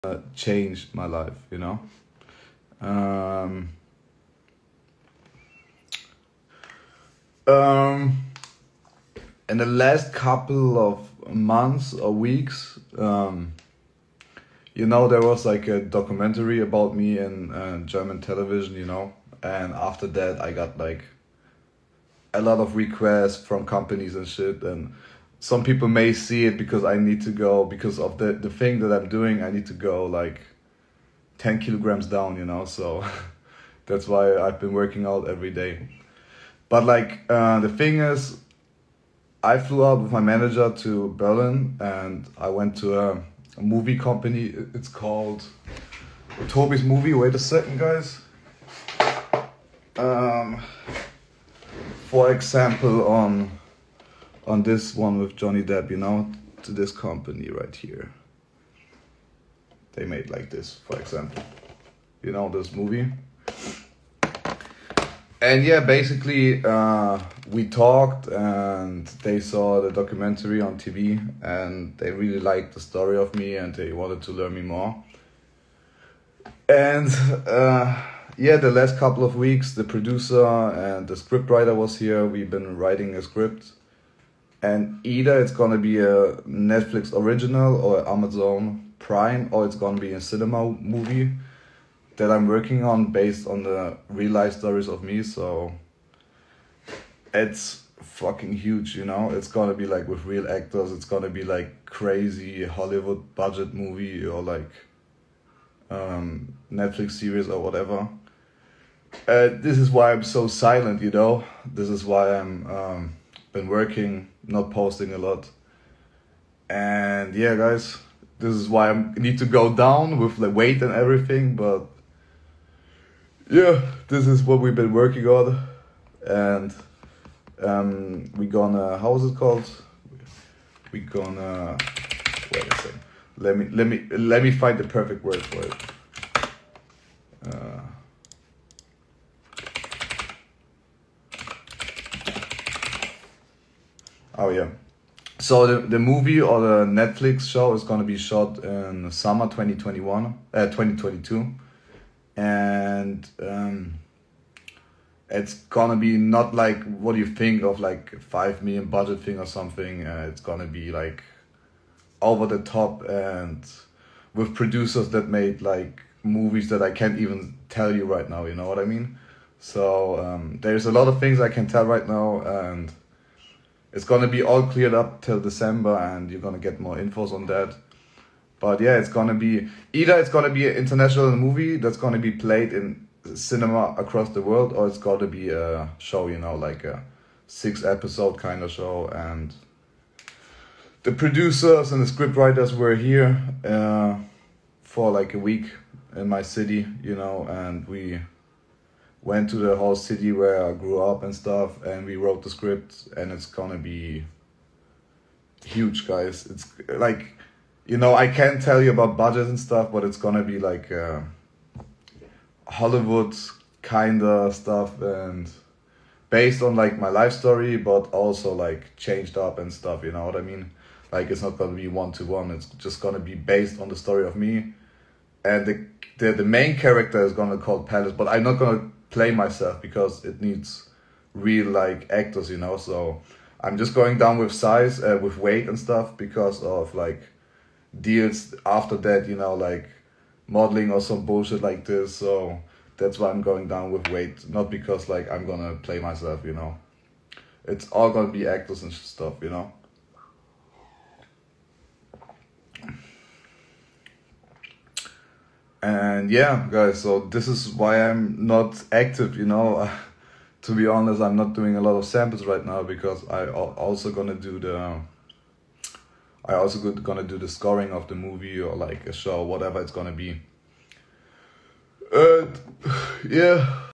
Why Palace been inactive lately BIG ANNOUNCEMENT MOVIE instagram live 14.11.21
First producer in Germany that get a film/documentary about him self with big film producers und very professional production, writing, actors etc